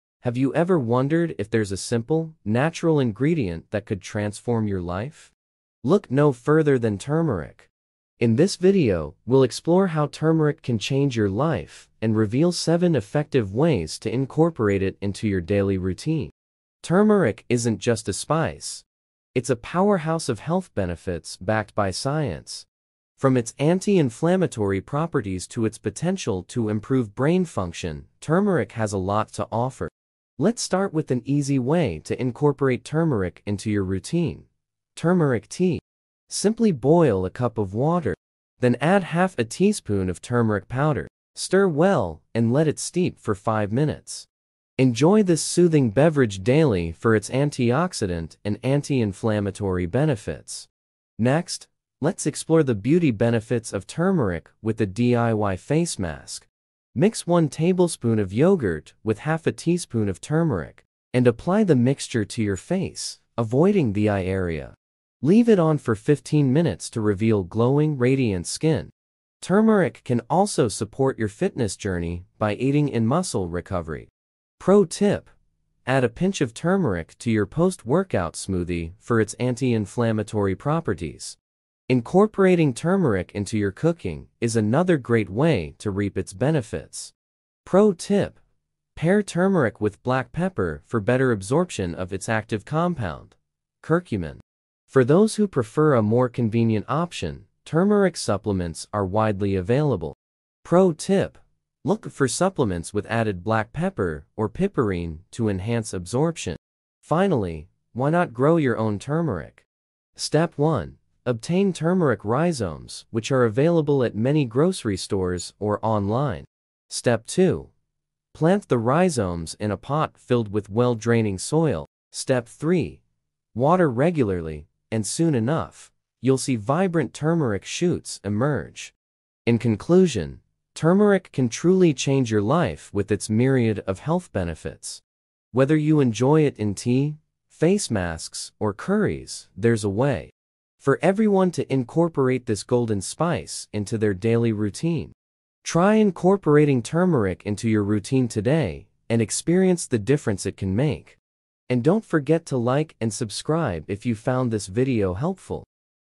Transform Your Health with Turmeric: 7 Ways to Use It
Discover how turmeric can change your life with these 7 versatile ways to use it! From adding flavor to dishes to soothing inflammation, turmeric is a powerful spice with numerous health benefits. Learn how to incorporate it into your daily routine and unlock its full potential.